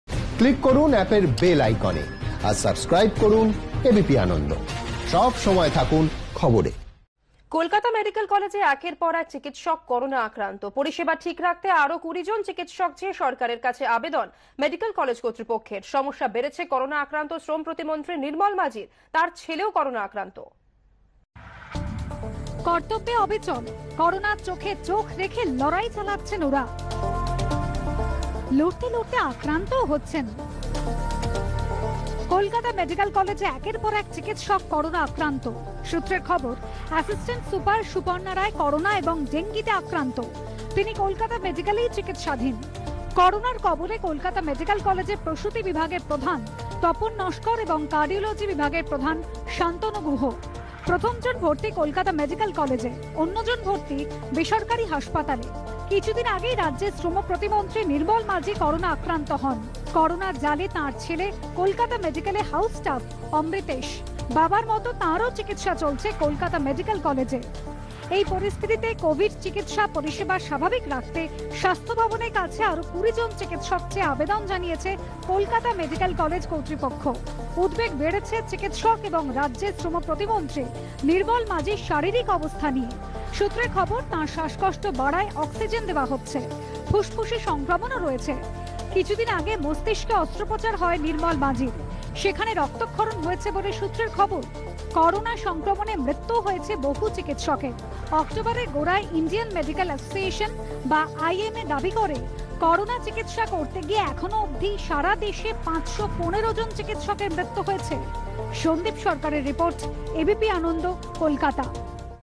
একের পর এক করোনা আক্রান্ত, সরকারের কাছে ২০ জন চিকিৎসক চেয়ে আবেদন কলকাতা মেডিক্যাল কলেজে কর্তৃপক্ষ
কলকাতা মেডিক্যাল কলেজে একের পর এক চিকিৎসক করোনা আক্রান্ত। পরিষেবা ঠিক রাখতে আরও ২০ জন চিকিৎসক চেয়ে সরকারের কাছে আবেদন মেডিক্যাল কলেজ কর্তৃপক্ষের। করোনার কবলে কলকাতা মেডিক্যাল কলেজের প্রসূতি বিভাগের প্রধান তপন নস্কর এবং কার্ডিওলজি বিভাগের প্রধান শান্তনু গুহ। সমস্যা বেড়েছে করোনা আক্রান্ত শ্রম প্রতিমন্ত্রী নির্মল মাজির। তাঁর ছেলেও করোনা আক্রান্ত। অ্যাসিস্ট্যান্ট সুপার সুপর্ণা রায় করোনা এবং ডেঙ্গিতে আক্রান্ত।